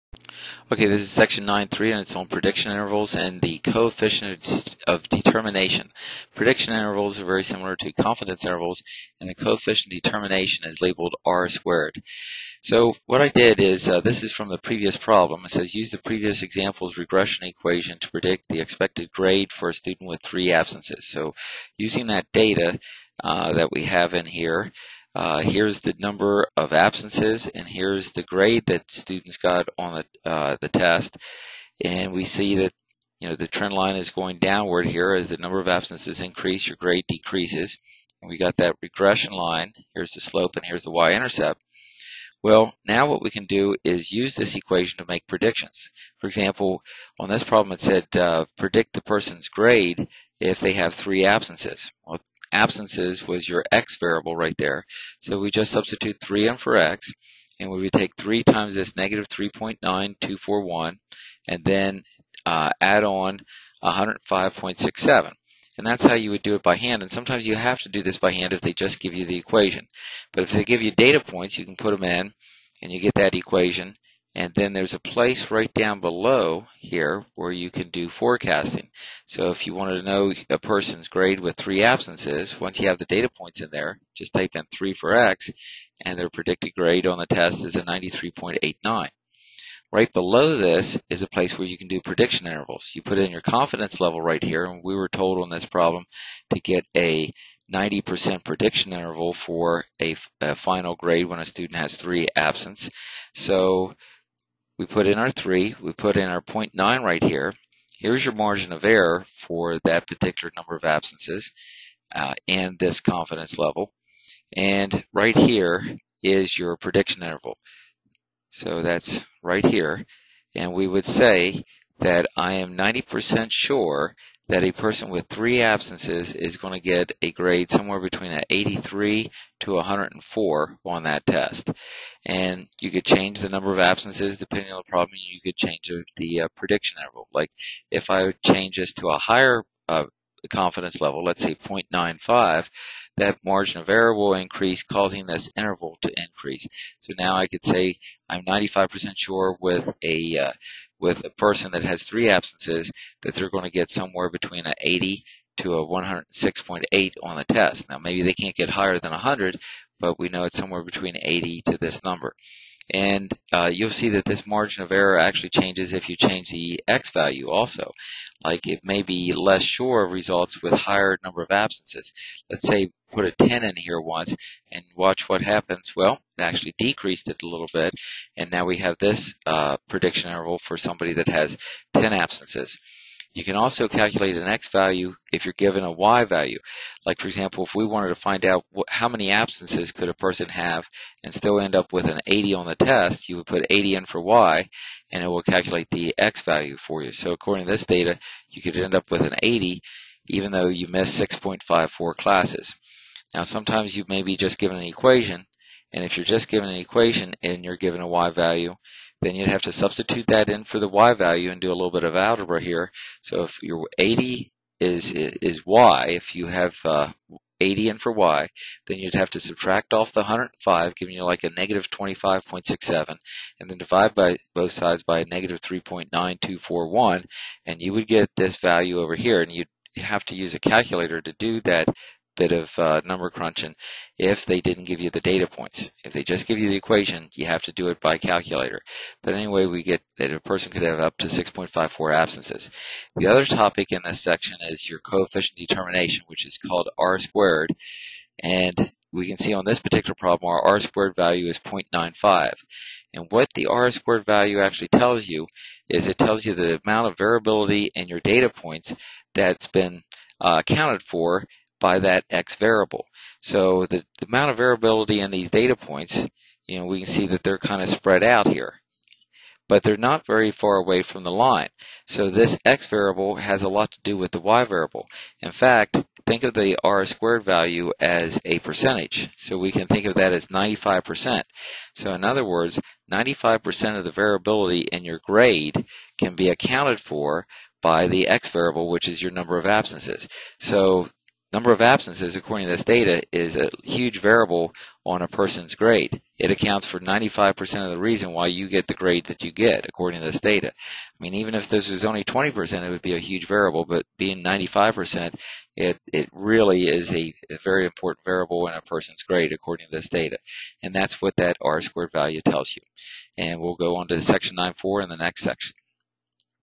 9_3 Predicting values, prediction intervals, coefficient of determination
Regression and Correlation Hypothesis tests, correlation coefficient, Predicting values, prediction intervals, coefficient of determination. and click on Excel sheets